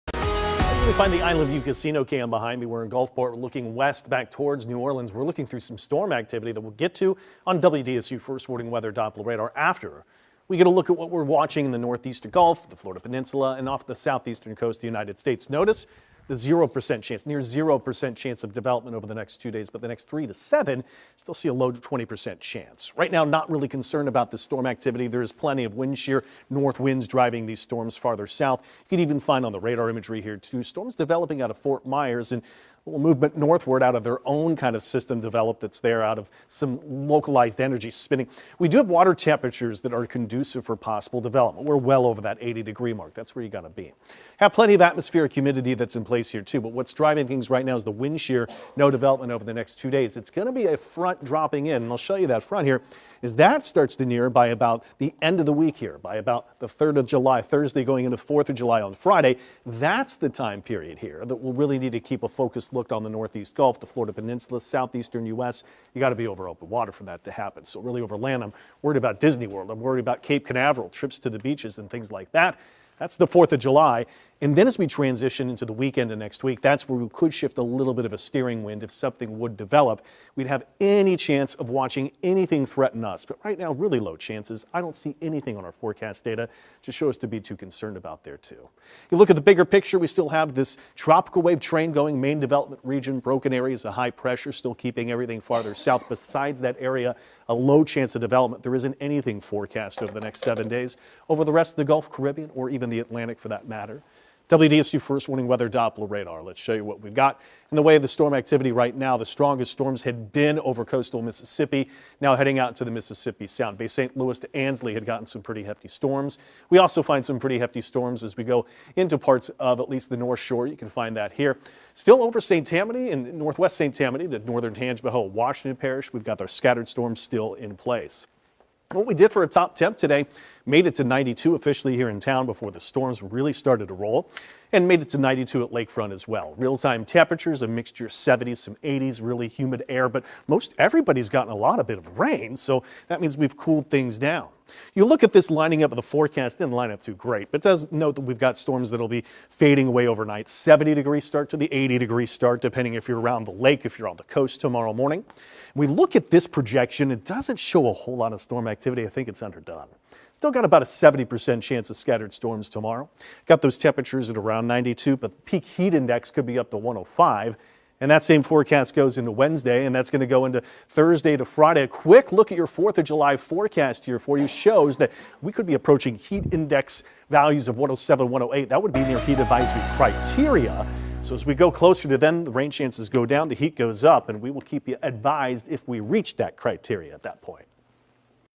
Tracking storms, tropics, and your 4th of July outlook in your New Orleans weather forecast
Meteorologist Devon Lucie starts with a look at the tropics where there's a low chance of tropical development over the next 3-7 days. He shows you what's keeping anything from developing over the next 2 days and what the extended forecast for days 3-7 looks like, especially if you've got travel plans to Florida beaches or attractions for the 4th of July. Devon then tracks the heaviest storms that were out at the time of the broadcast and gets right to the high resolution forecast showing you when an where storms are likeliest on Tuesday and how hot a humid it gets and feels before finishing with your seven day outlook.